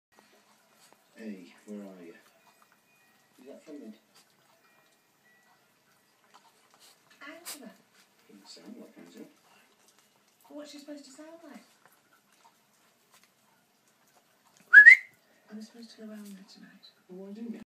cat loving jackshit
bertie loves his cat and his cat luvs him!! sorry about the annoying tv in the back ground... humans huh?